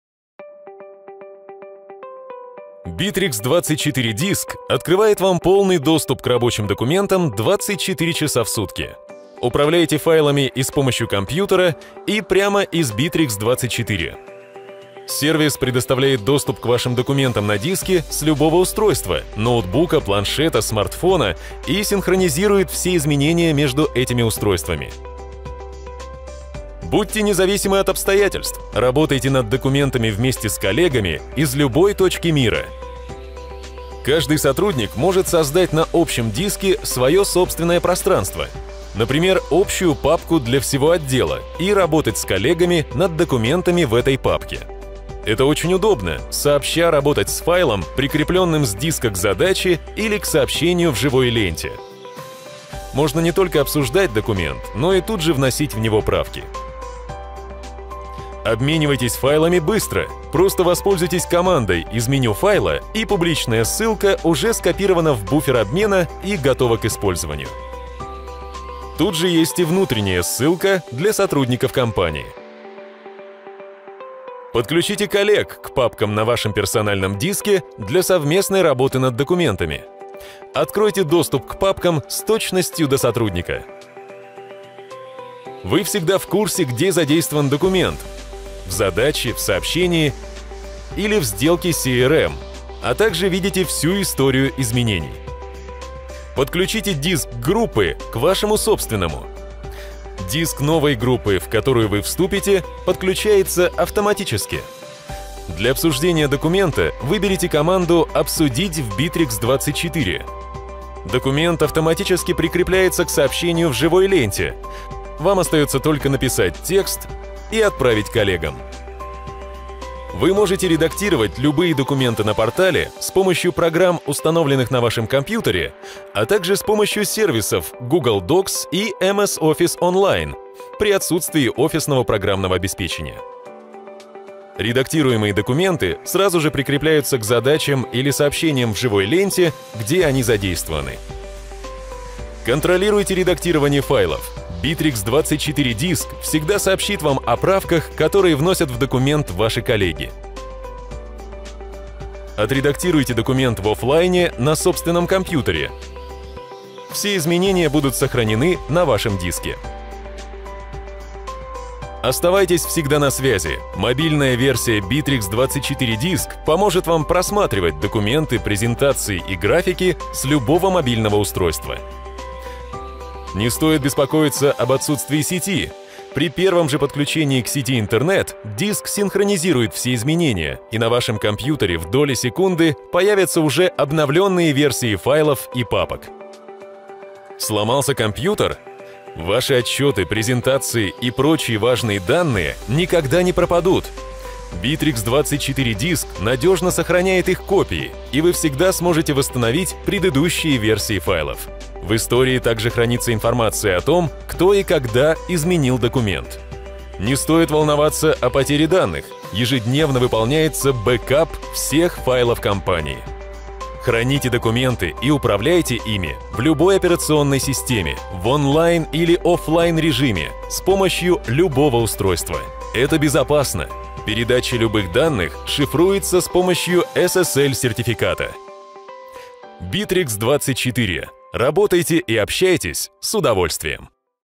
Битрикс24.Диск
Доступ ко всем рабочим файлам 24 часа в сутки с любого устройства и синхронизирует все данные между этими устройствами.
Обсуждайте, обменивайтесь и меняйте рабочие файлы для совместной
работы над документами